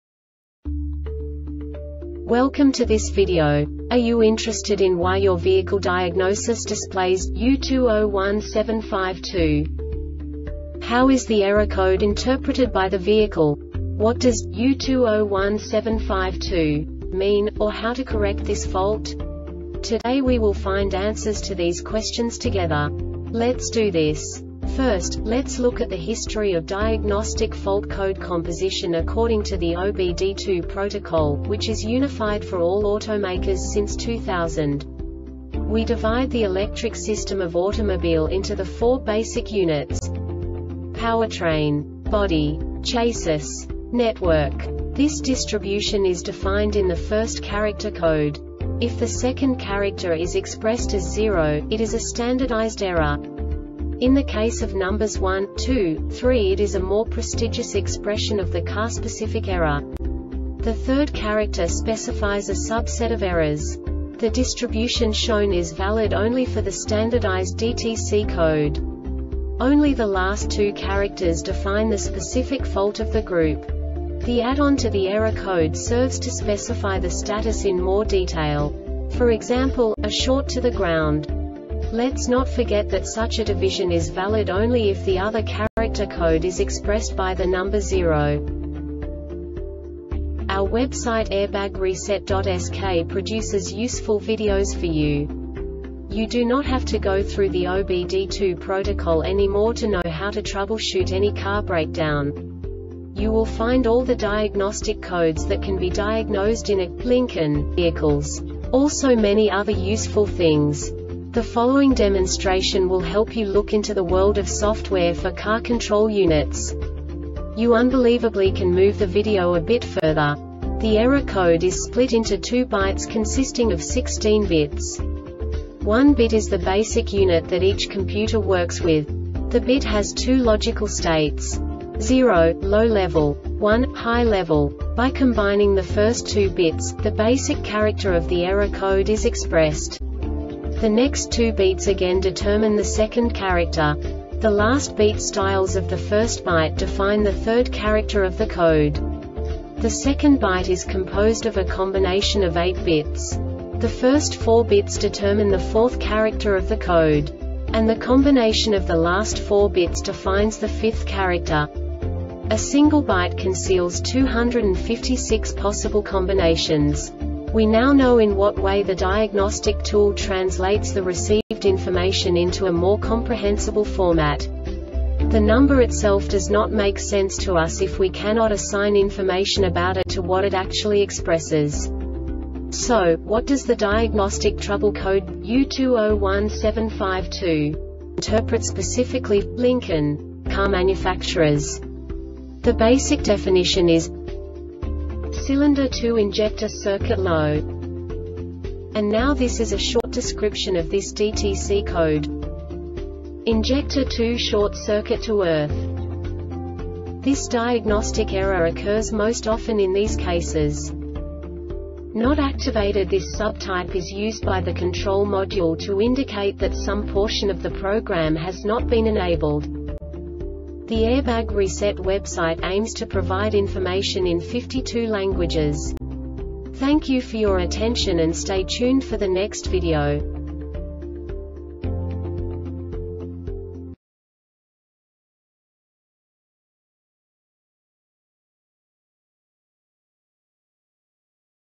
DTC Lincoln U2017-52 Short Explanation
Contents:
0:21 Basic DTC analysis according to OBD2 protocol standard.
1:48 Insight into programming
2:58 A diagnostically understandable description of the Diagnostic Trouble Code
3:05 Basic error definition

Make:
Lincoln
Code:
U2017 52
Definition:
Cylinder 2 injector circuit low
Description:
Injector 2 short circuit to earth
Failure Type:
Not Activated
This sub type is used by the control module to indicate that some portion of the program has not been enabled.